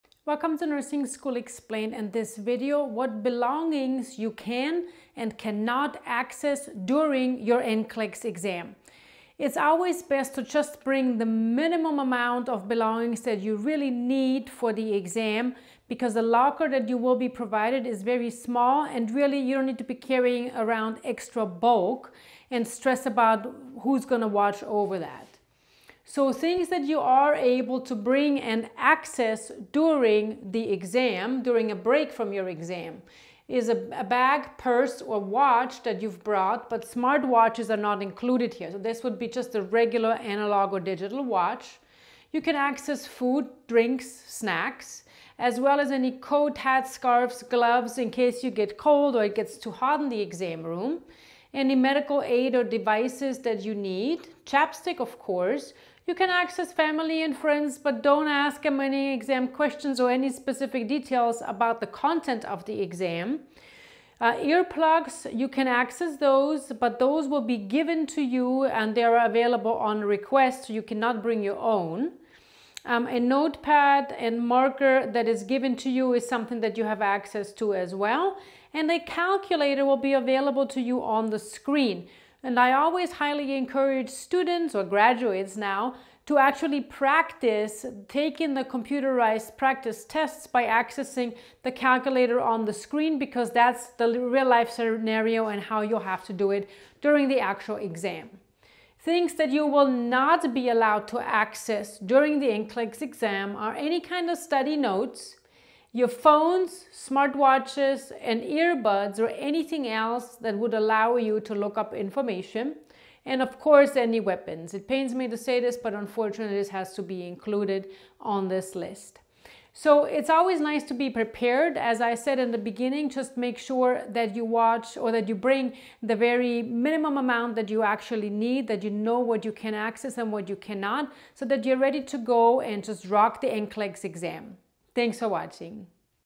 Can I Access my Belongings during NCLEX
This video explains what belongings you can and cannot access during your NCLEX exam.